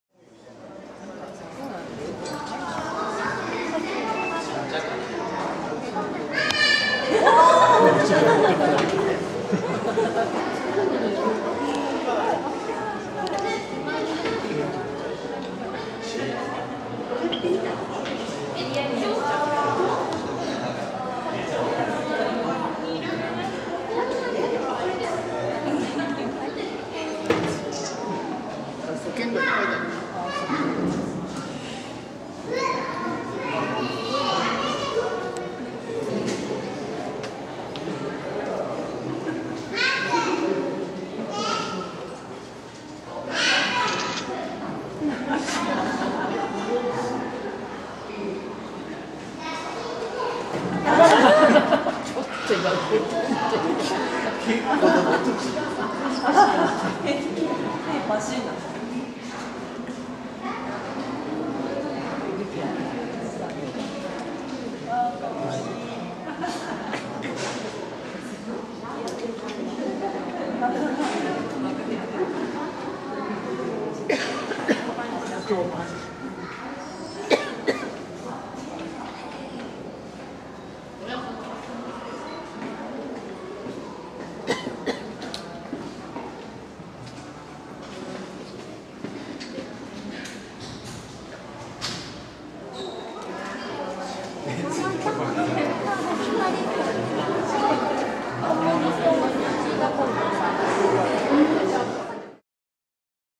リーベの背中に乗るリアン~Snow Leopard run,jump,play
円山動物園のユキヒョウ、リーベ(メス/8歳）と、リアン(メス/2011年5月1日生まれ)。
走る、ジャンプ、じゃれて遊ぶの親子。
(2011年8月20日撮影)